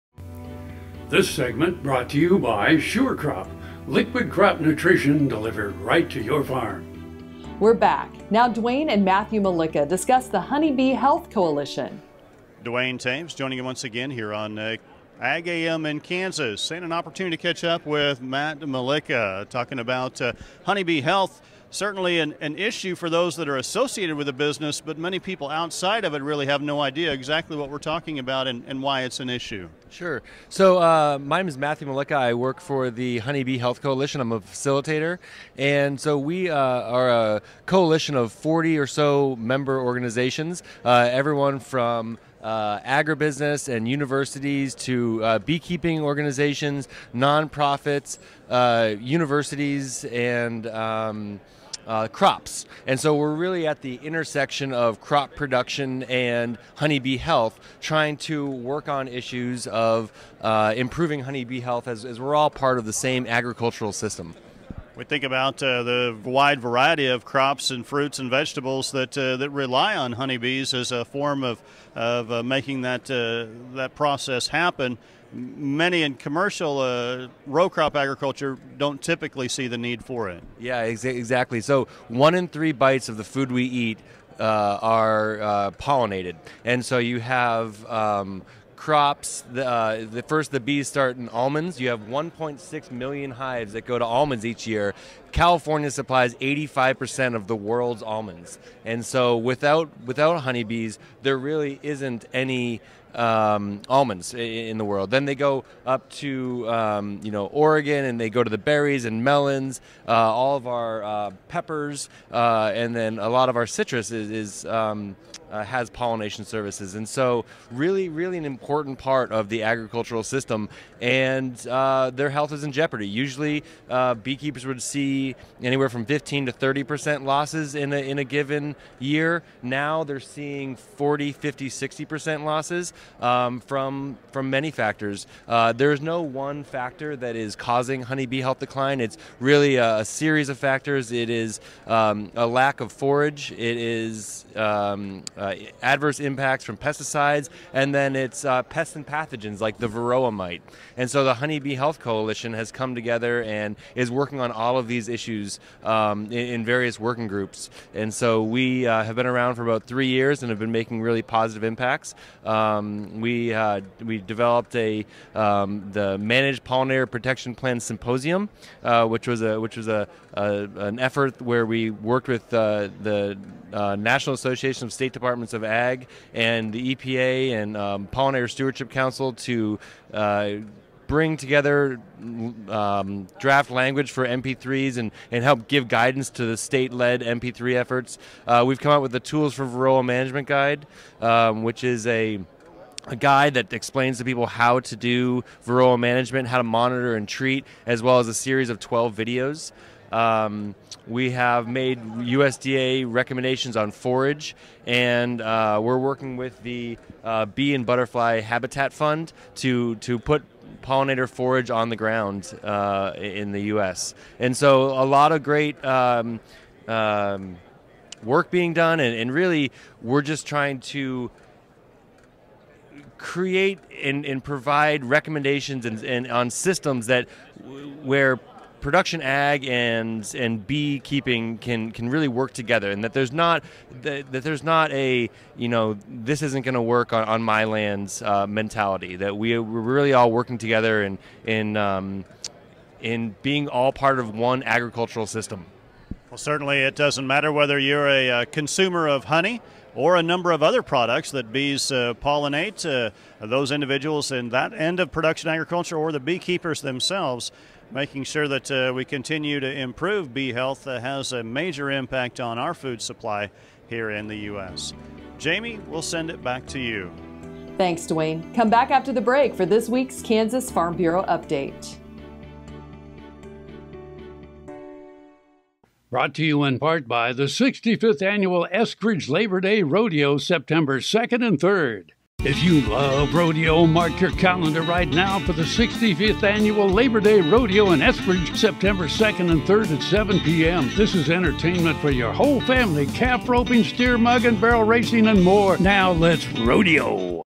Farm Factor - Matt Mulicia Talks About the Importance of Bees -August 22, 2017
We're back! Now Duane and Matthew discuss Bees. (Duane Toews) Duane Toews joining you once again here on AGam in Kansas. an opportunity to catch up with Matthew Mulica talking about Honey Bee Health. Certainly an issue for those that are associated with the business but many people outside of it really have no idea exactly what we're talking about and why it's an issue.